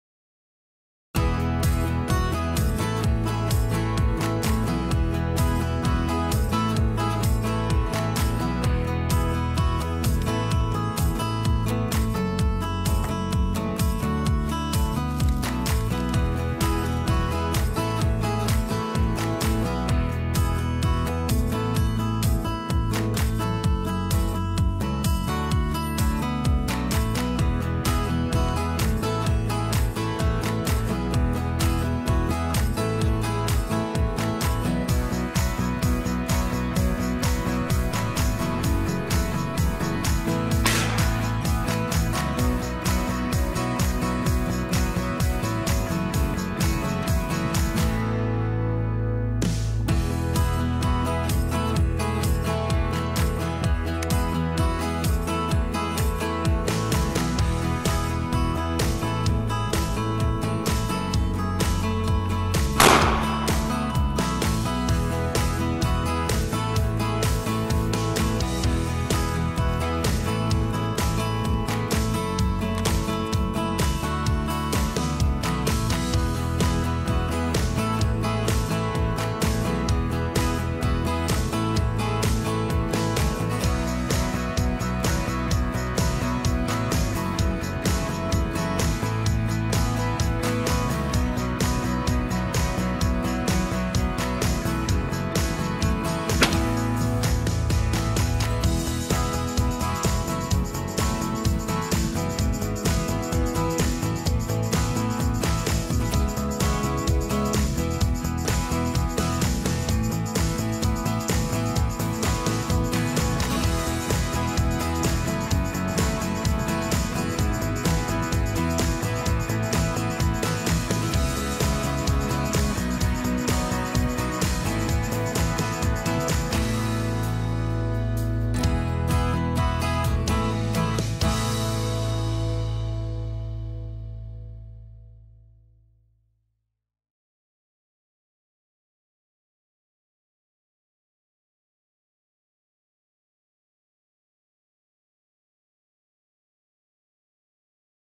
lov tetřivka
Lov tetřívků ve Finsku novým kulobrokem z České zbrojovky CZ Combo